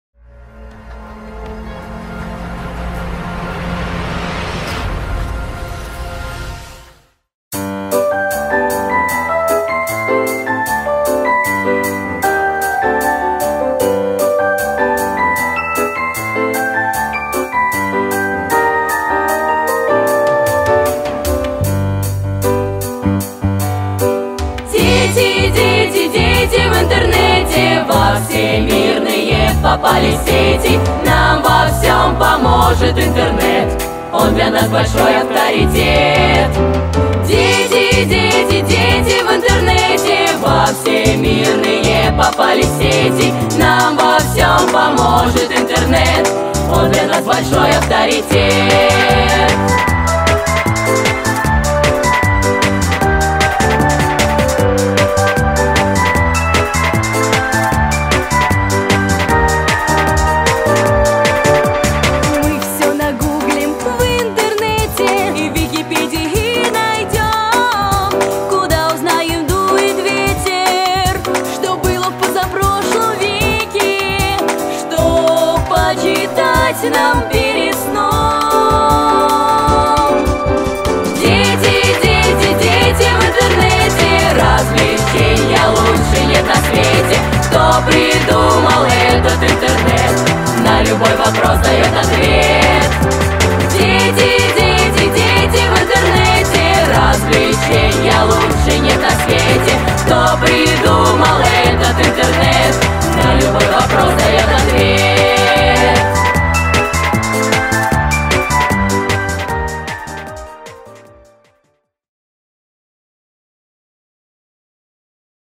Дети в интернете
Трейлер конкурсного спектакля "Дети в интернете" (театр "Мюзикл", ДШИ им. А. Н. Верстовского), автор - Ольга Панаетова (медиа-проект АRTИСТ). XII Международный фестиваль любительских театров "Молодые - молодым", Москва, ЦК "Сцена", 2015.